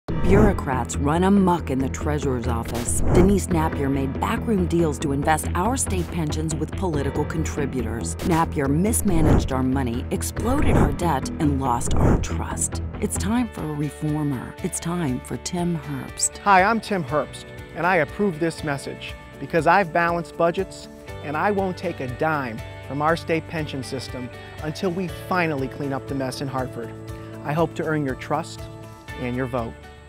"Amok"
Bureaucrats run amok in the Treasurers office, Denise Nappier made back room deals to invest our state pensions with political contributors. Nappier mismanaged our money, exploded our debt and lost our trust. It’s time for a reformer, its time for Tim Herbst.

Hi I’m Tim Herbst and I approve this message because I’ve balanced budgets and I won’t take a dime from the state pension system until we finally clean up the mess in Hartford. I hope to earn your trust, and your vote.